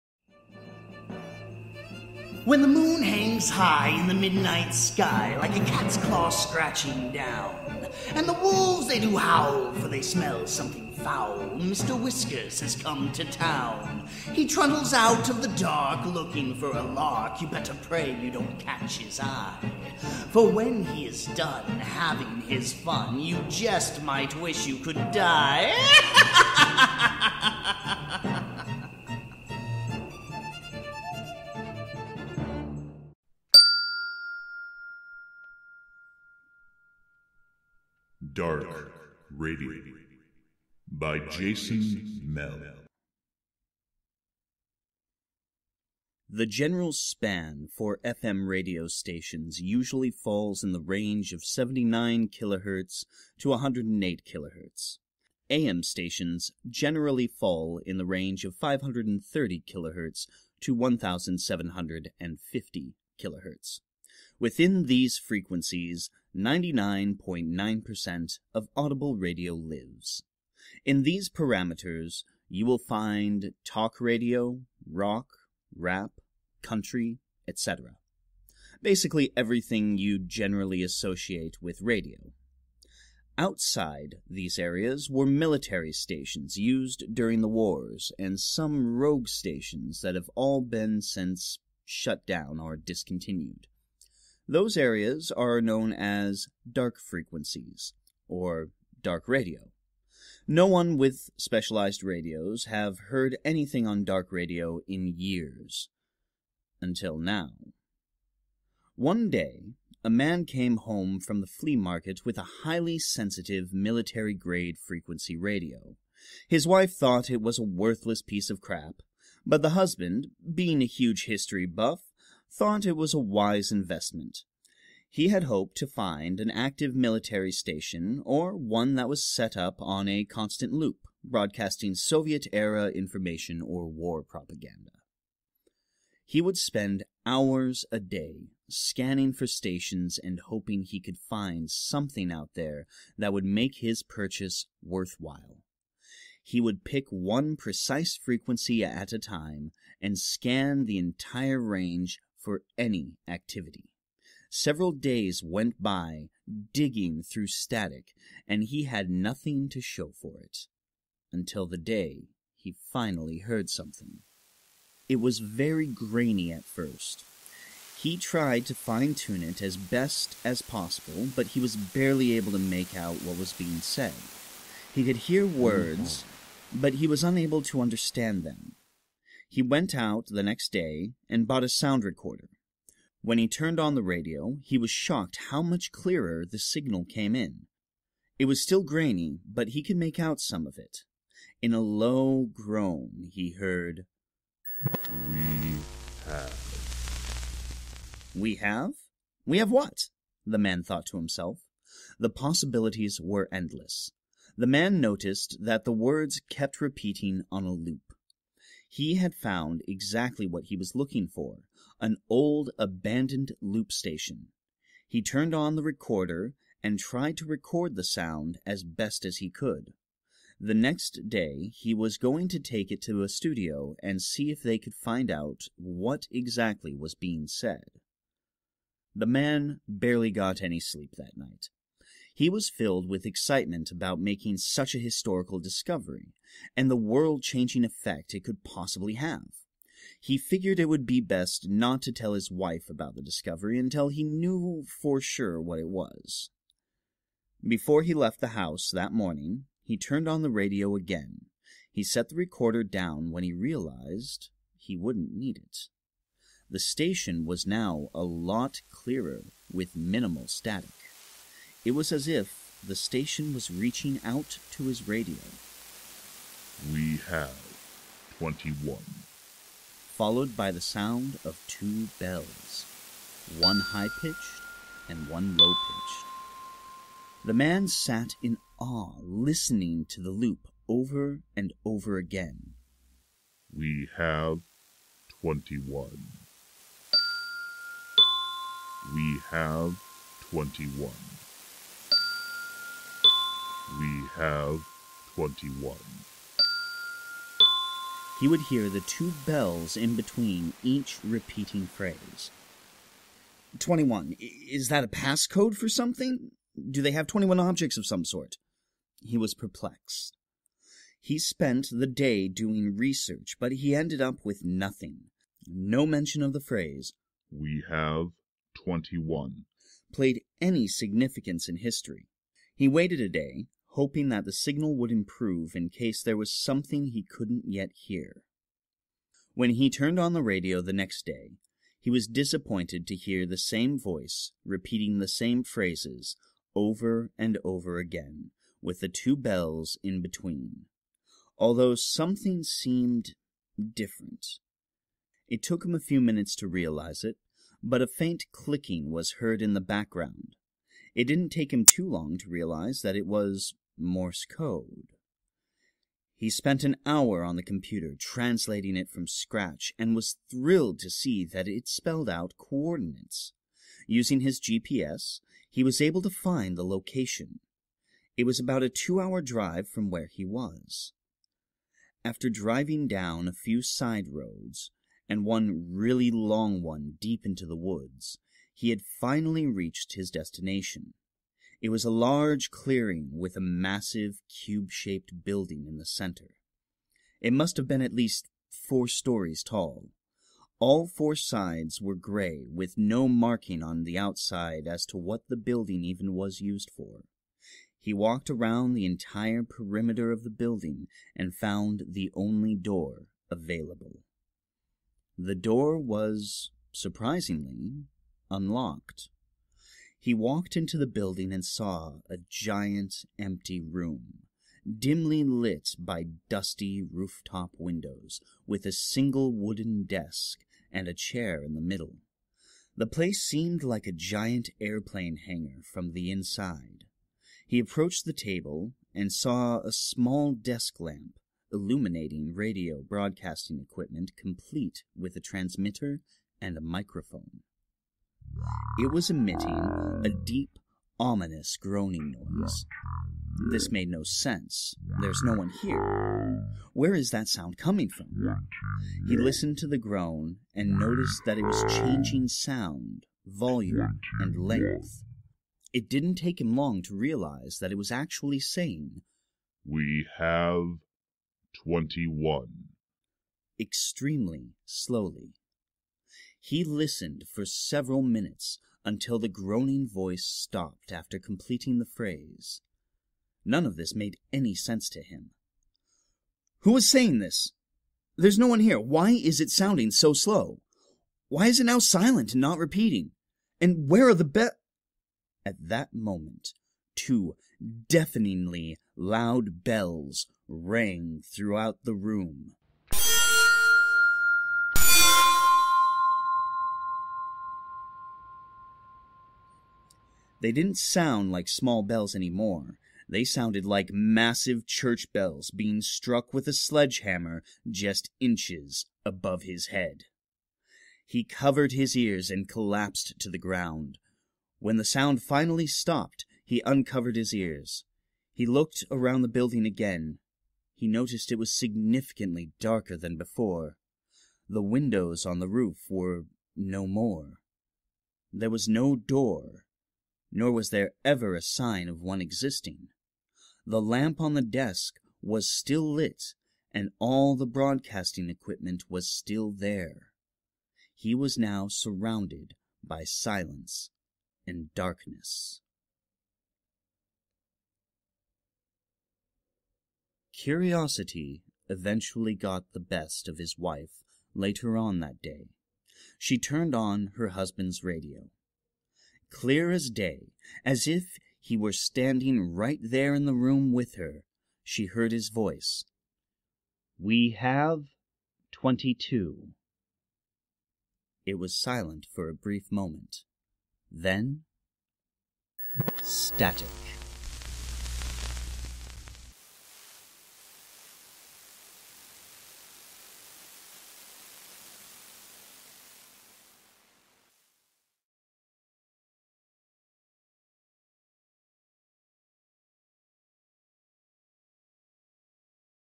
Dark Radio
What happens when a man digs a little too deep into a mysterious radio signal? Find out as we hear what happens when he gets a "Dark Radio"